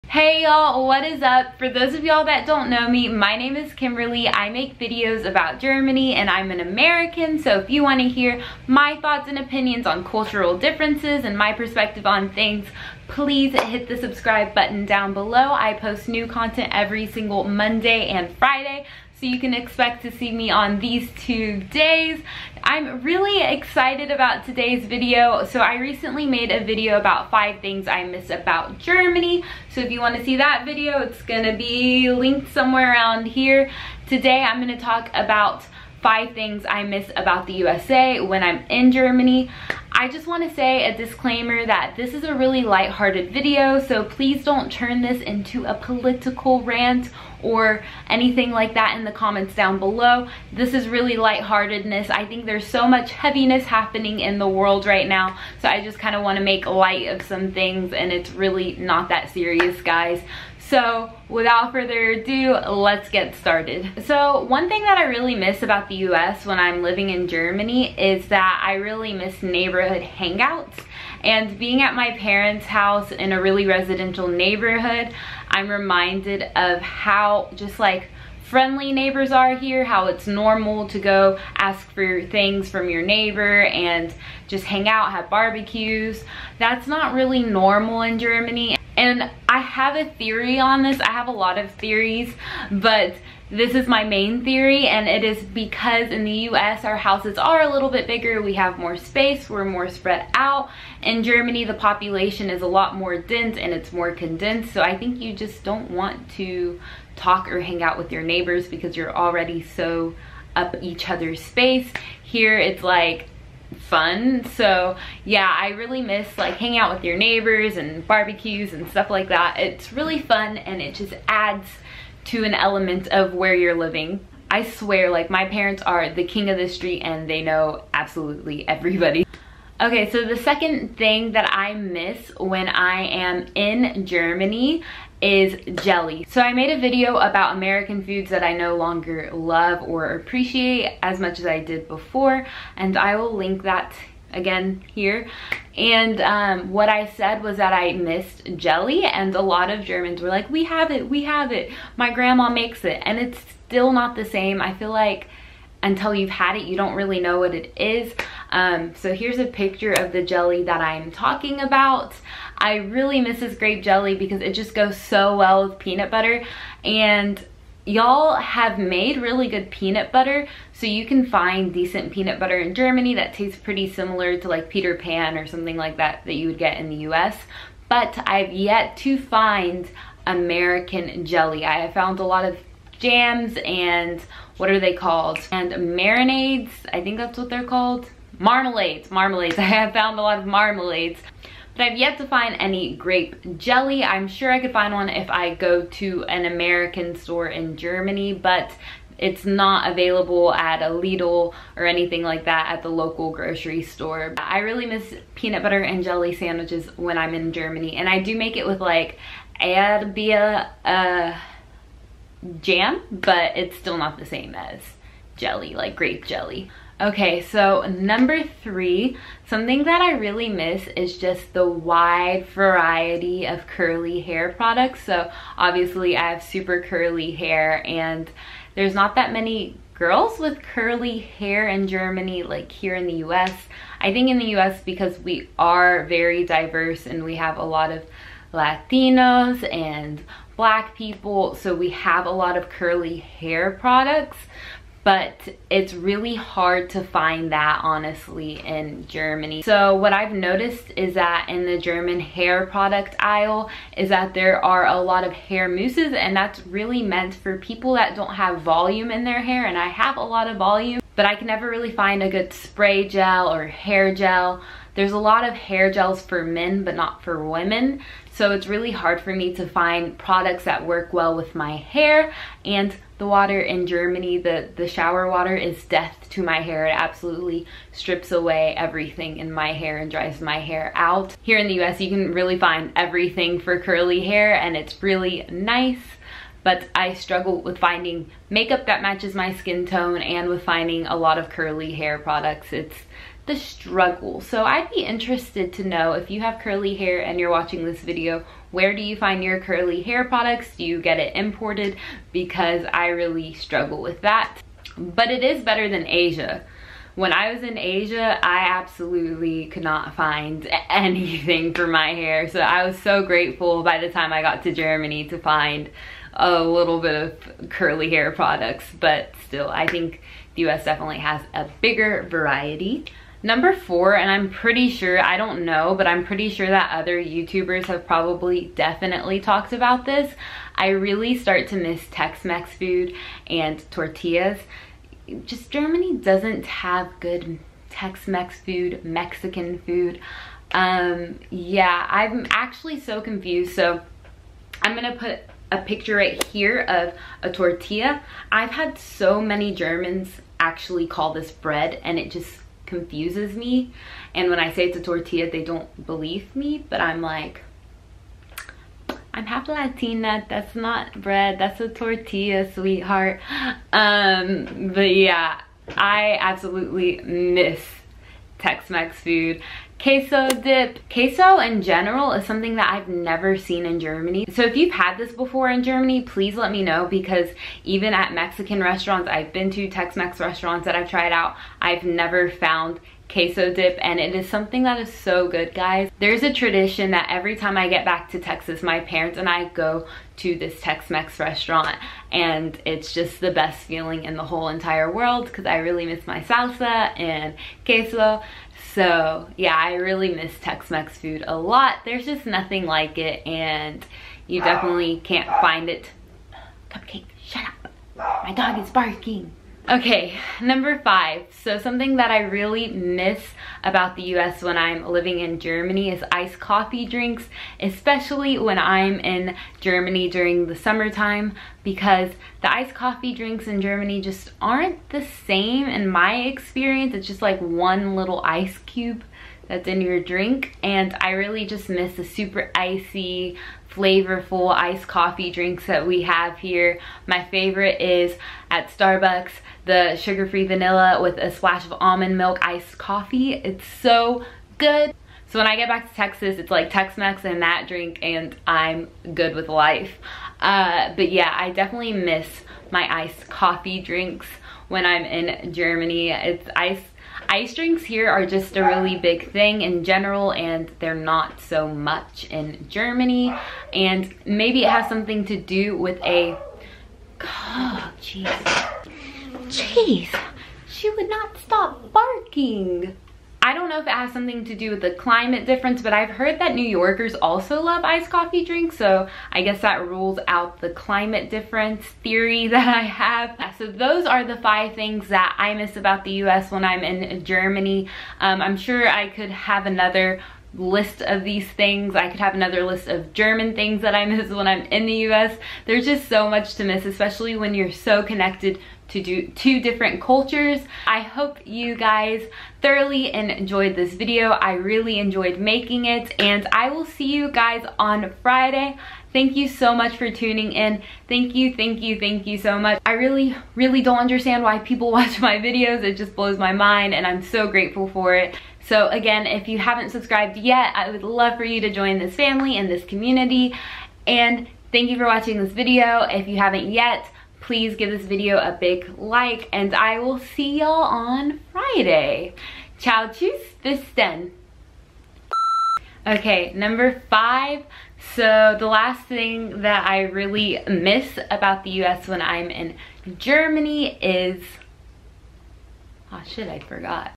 5 Things I cannot find in Germany (as an American)
Did y'all notice my mood is meh? I am tired of staying inside. I am ready to go out and explore and see people and move on with my life. Like all of us are. Lol! But I hope you guys enjoyed this video as always. I am still doing my underrated city video but I am pushing it back because I want to prepare better for it.

I am currently trying to upgrade my equipment and get a microphone to help my sound quality.
LET"S GET SOCIAL
My Camera